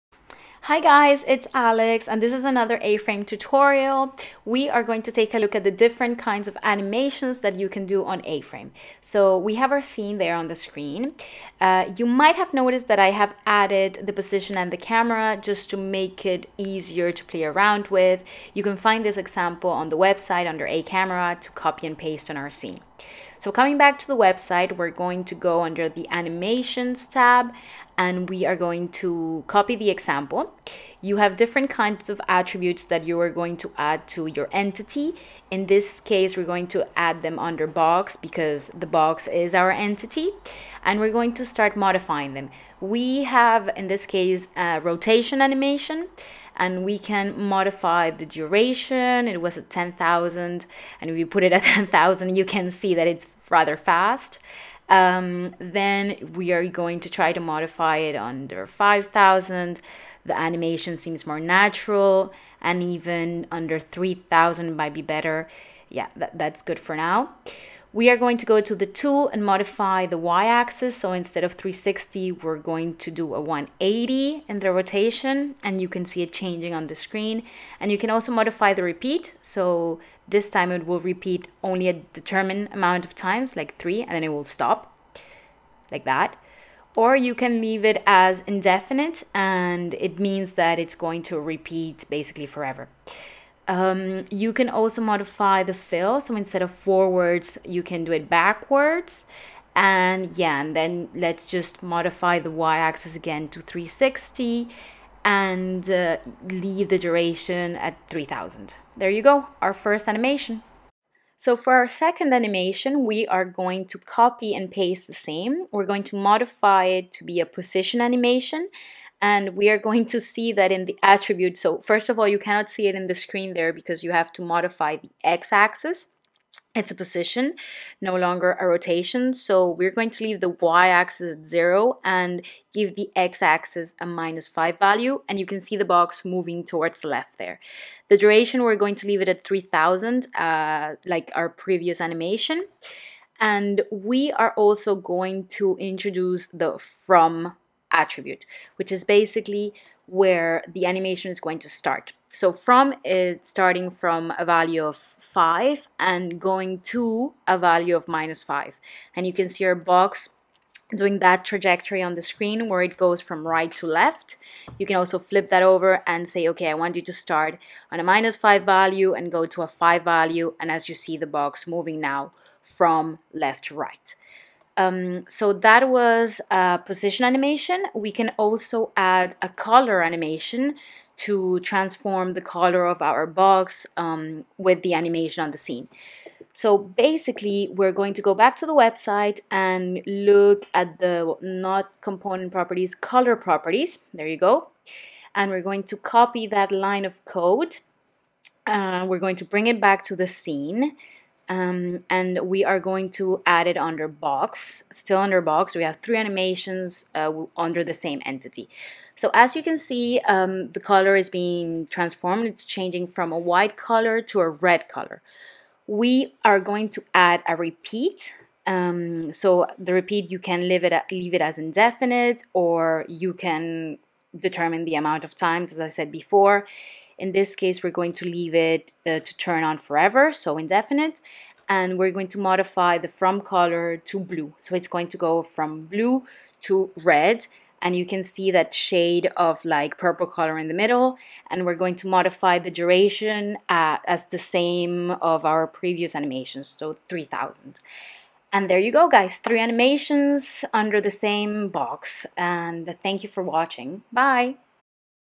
A-Frame Animation Tutorial
This is a basic beginner tutorial on how to do animations with A-frame from Mozilla VR. This example was done in Codepen. We are going to see how to do rotation, position and color animations.